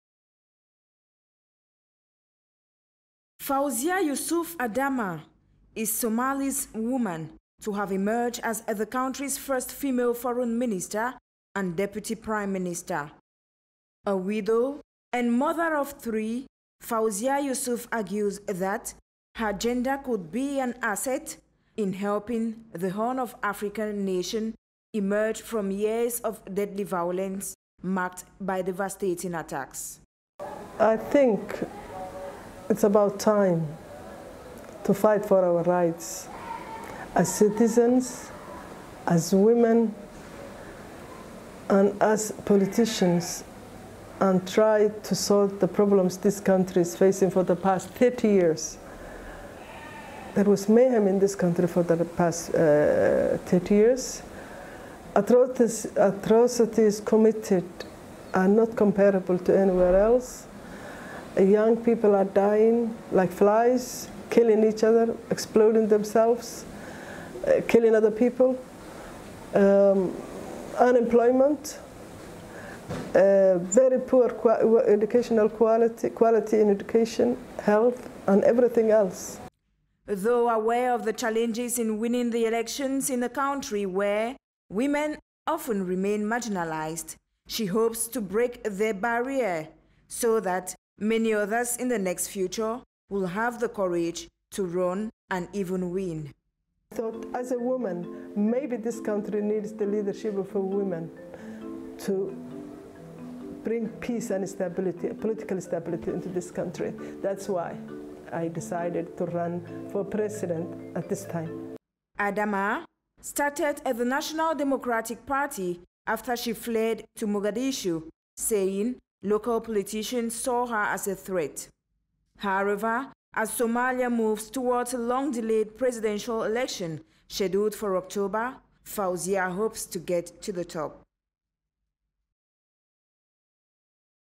FAWZIA YUSUF ADAM, FIRST SOMALI WOMAN TO RUN FOR PRESIDENT
Somalia's female lawmaker Fawzia Yusuf Adam has announced she'll be standing as a presidential candidate in the country's upcoming elections. Subiru Madina tells us in the following report that, she argues her gender could be an asset in helping Somalia emerge from years of deadly attacks.